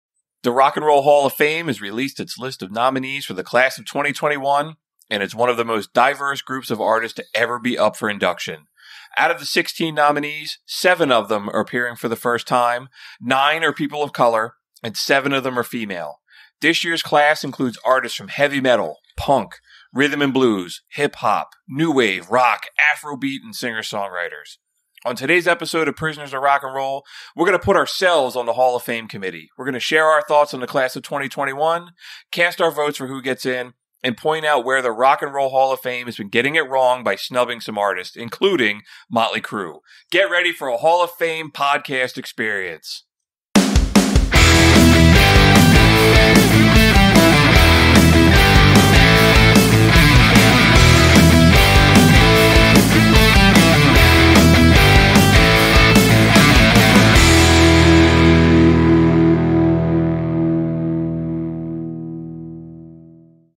TRAILER -- What the Rock and Roll HOF Got Wrong in 2020
The Rock and Roll Hall of Fame has released the list of nominees for the class of 2021, and it’s one of the most diverse groups of artists to ever be up for induction, including 7 first time nominees, 9 people of color, and 7 women.

The artists are from heavy metal, punk, R&B, hip hop, new wave, rock, afrobeat, and singer-songwriters. In this episode, we share our thoughts on the class of 2021, cast our votes for who gets in, and point out where the Hall has been getting it wrong by snubbing some artists (including Motley Crue). Get ready for a hall of fame podcast experience!